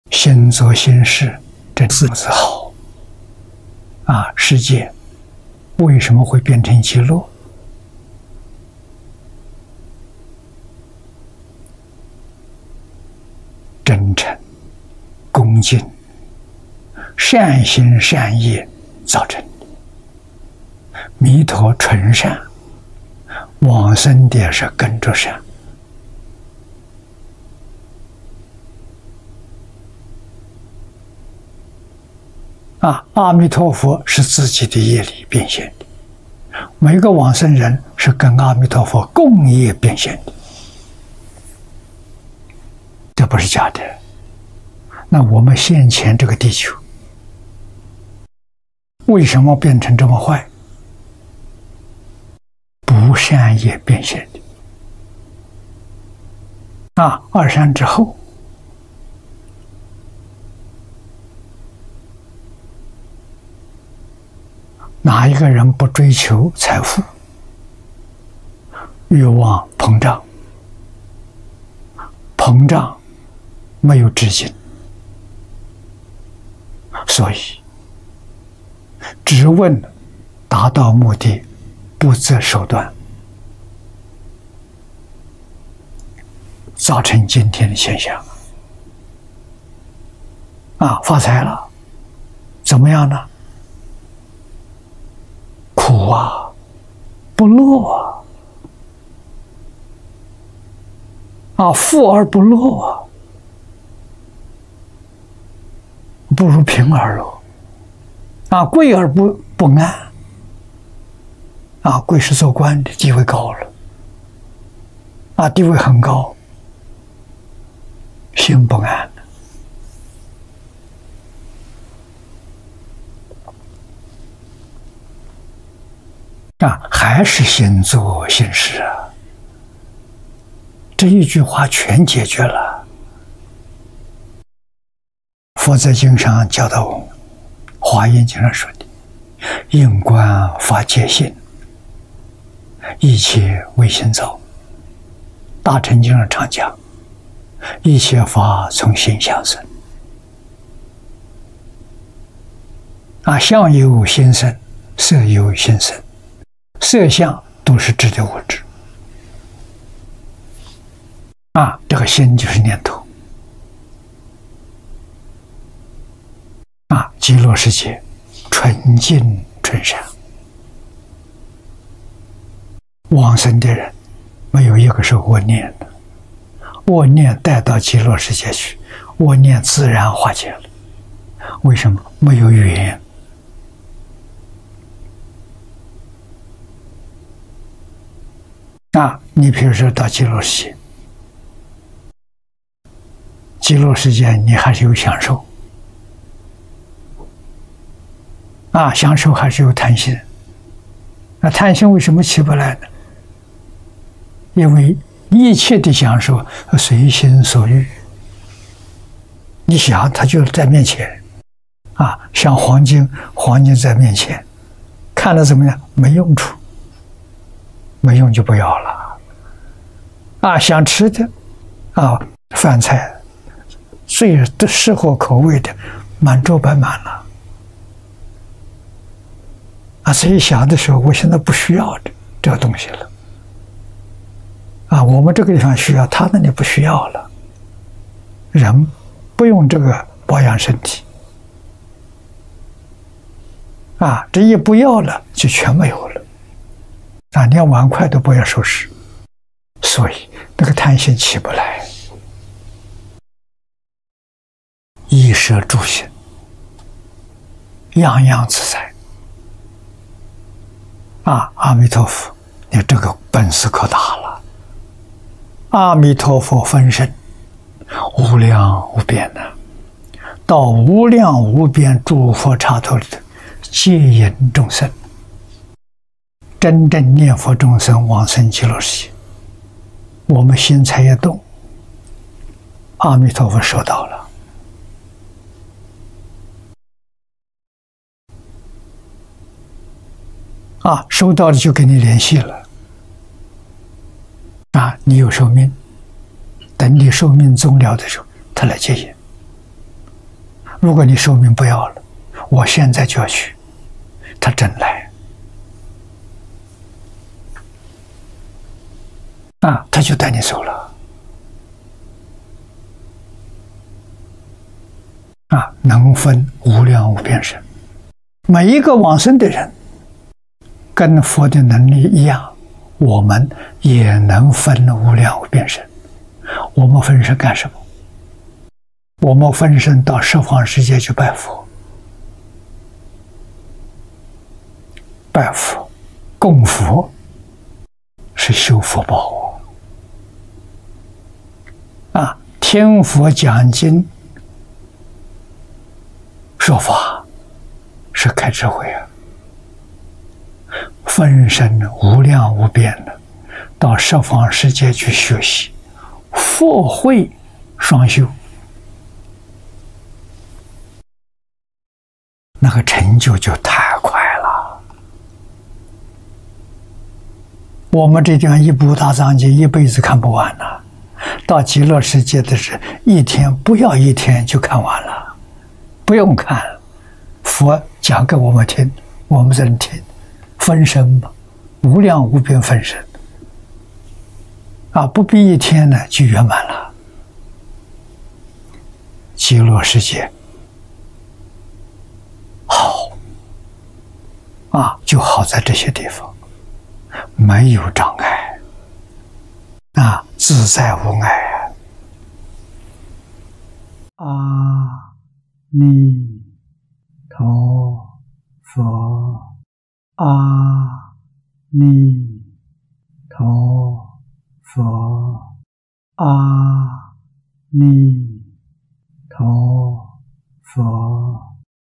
🙏淨空老法師🙏你看這個本事可大了 !🙏
「心作心是」，這四個字好，世界為什麼會變成極樂？真誠、恭敬、善心善業造成的。彌陀純善，往生的也是跟著善，阿彌陀佛是自己的業力變現的，每個往生的人是跟阿彌陀佛共業變現的，這不是假的。我們現前這個地球為什麼變成這麼壞？不善業變現的。二戰之後，哪一個人不追求財富？欲望膨脹，膨脹沒有止境，所以只問達到目的，不擇手段，造成今天的現象。發財了，怎麼樣？苦，不樂，富而不樂，不如貧而樂。貴而不安，貴是做官的，地位高了，地位很高，心不安。還是心作心是，這一句話全解決了。佛在經上教導我們，《華嚴經》上說的，「應觀法界性，一切唯心造」，大乘經上常講，「一切法從心想生」、「相由心生，色由心生」，色相都是指的物質，這個心就是念頭。
　　極樂世界純淨純善，往生的人沒有一個是惡念，惡念帶到極樂世界去，惡念自然化解了，為什麼？沒有緣。到極樂世界，極樂世界你還是有享受，享受還是有貪心，貪心為什麼起不來？因為一切的享受都隨心所欲，你想它就在面前。想黃金，黃金在面前，看了怎麼樣？沒用處，沒用就不要了。想吃的，飯菜，最適合口味的，滿桌擺滿了；再一想，我現在不需要這個東西了，我們這個地方需要，它那裡不需要，人不用這個保養身體，這一不要，就全沒有了，連碗筷都不要收拾。所以那個貪心起不來，衣食住行樣樣自在。
　　阿彌陀佛，你看這個本事可大了，阿彌陀佛分身無量無邊，到無量無邊諸佛剎土裡頭接引眾生。真正念佛眾生往生極樂世界，我們心才一動，阿彌陀佛收到了，收到就跟你聯繫，你有壽命，等你壽命終了的時候，他來接引；如果你壽命不要了，我現在就要去，他真來，他就帶你走了，能分無量無邊身。每一個往生的人跟佛的能力一樣，我們也能分無量無邊身，我們分身幹什麼？我們分身到十方世界去拜佛，拜佛、供佛是修福報，聽佛講經說法是開智慧。分身無量無邊，到十方世界去學習，福慧雙修，那個成就就太快了。我們這個地方一部《大藏經》一輩子看不完，到極樂世界，一天，不要一天就看完了；不用看，佛講給我們聽，我們在那裡聽，分身，無量無邊分身，不必一天就圓滿。極樂世界好就好在這些地方，沒有障礙，自在無礙。